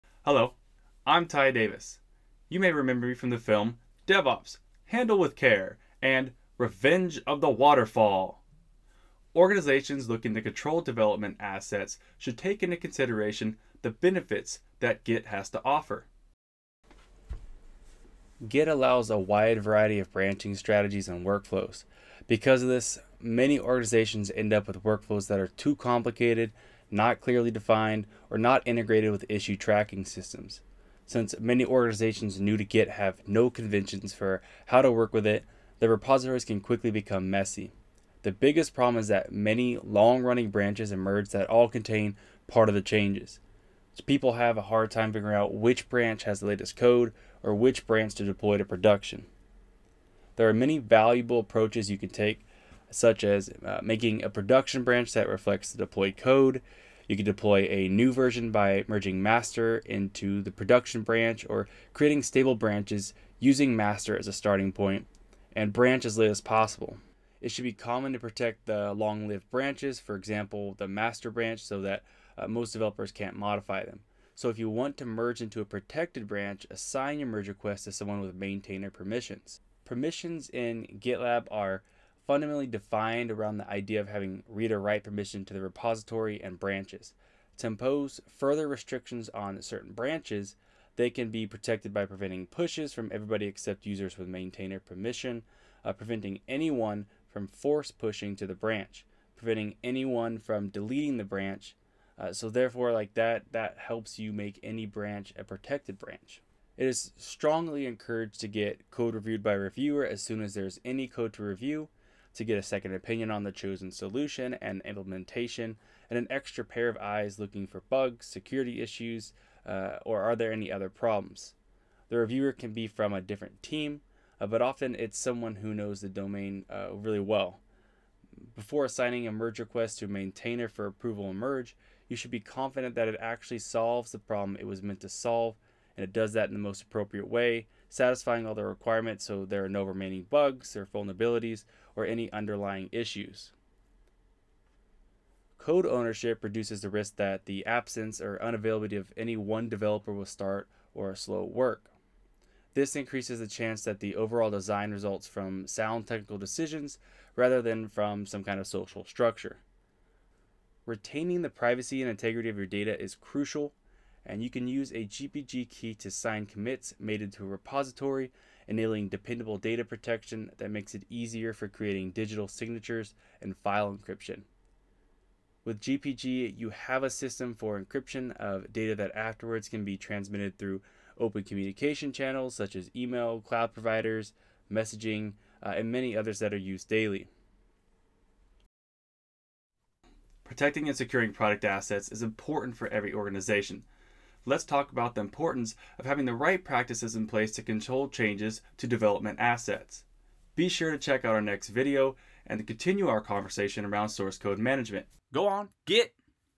VC&C - Control Changes to Product Development Assets
Version Control & Collaboration - Control Changes to Product Development Assets

Accelerate development, avoid confusion, and reduce rework by tracking and managing all your project assets. Development teams need a plan and a process to keep track of all the critical resources, files, and assets needed during planning, development, and delivery. Without a clear plan about how you’re going to manage these product assets, there’s a real risk that the wrong files, the wrong code, or wrong configuration can derail your project.